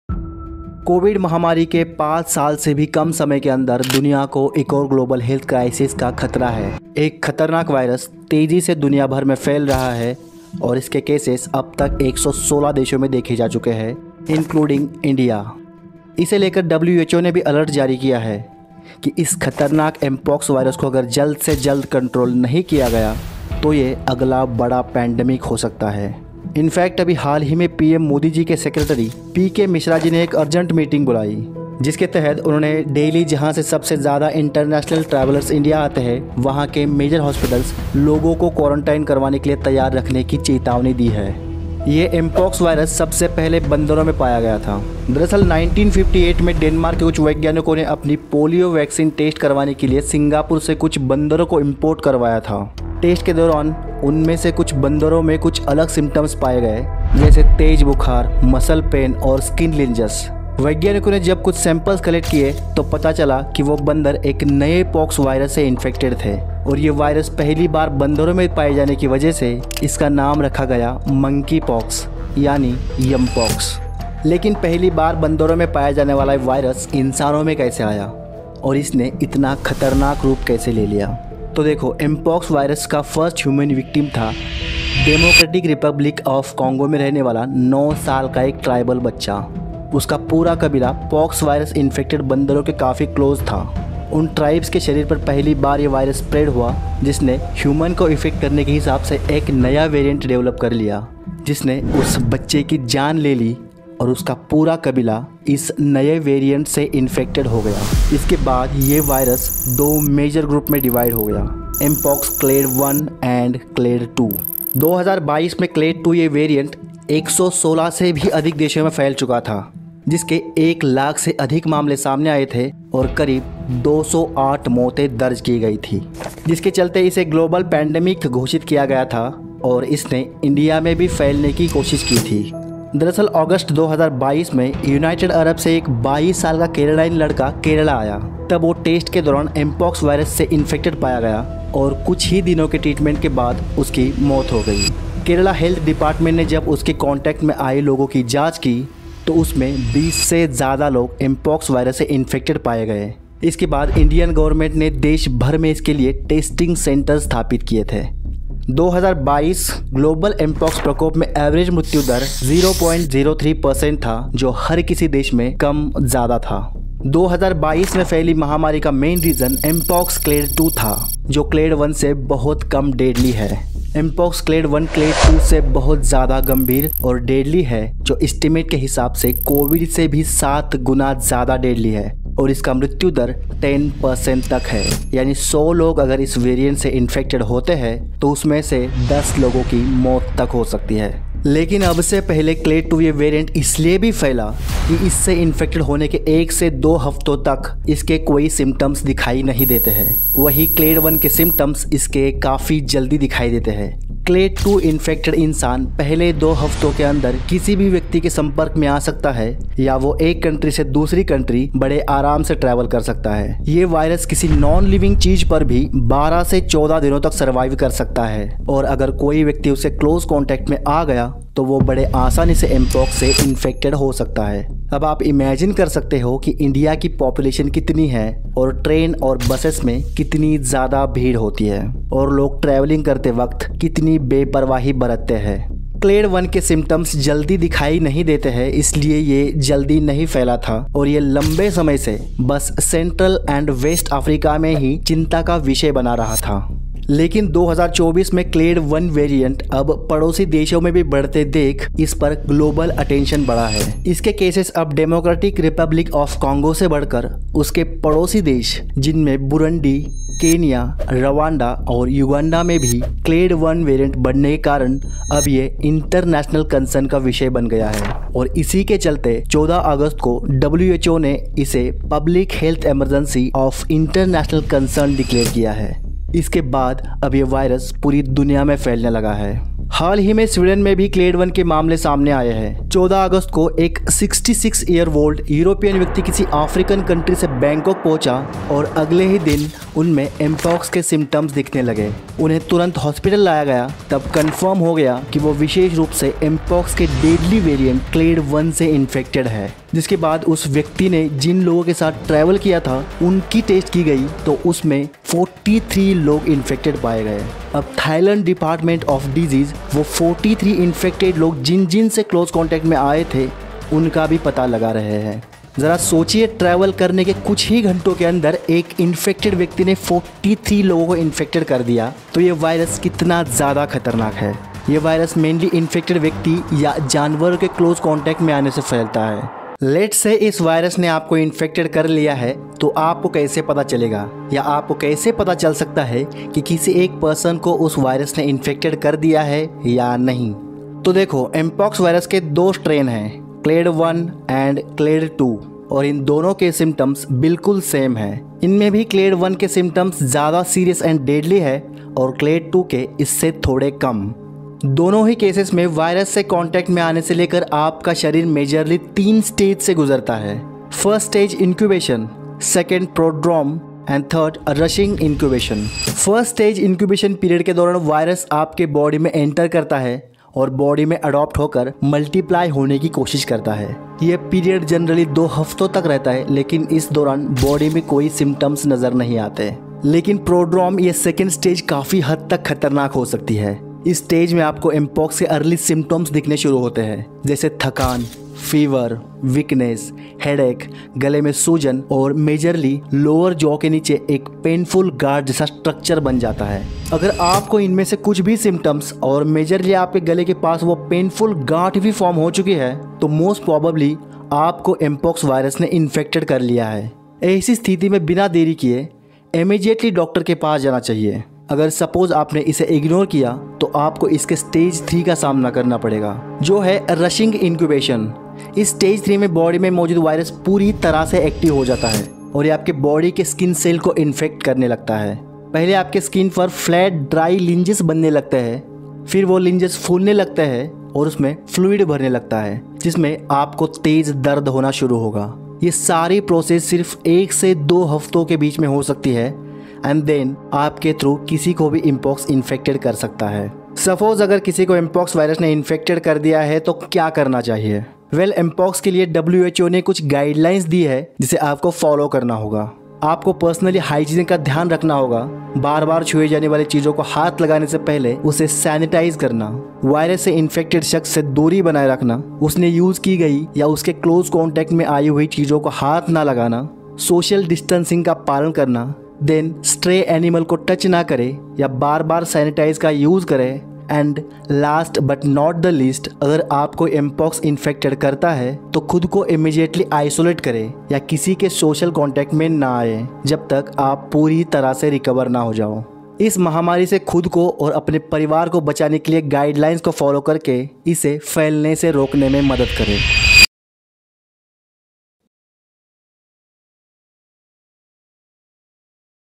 ALERT! The Rising Threat of MPOX // Mpox Virus Explained.
In under five years since the CVD 19: pandemic, the world faces another potential global health crisis: the MPOX virus. Spreading rapidly across 116 countries, including India, this virus has prompted the WHO to issue a critical alert. If not controlled quickly, MPOX could become the next major pandemic.

 In this video, we delve into the origins of MPOX, which was first identified in monkeys in 1958 and later found to infect humans. We explore the spread of this virus, particularly focusing on the more dangerous Clade 1 variant, which is seven times deadlier than CVD-19, with a mortality rate of up to 10%.

We discuss the current situation, including recent outbreaks in Europe and Africa, and what measures are being taken globally to contain it. Learn about the symptoms of MPOX, the stages of infection, and crucial preventive measures to protect yourself and your loved ones.

Stay informed and stay safe by understanding the nature of this emerging threat and following the recommended guidelines.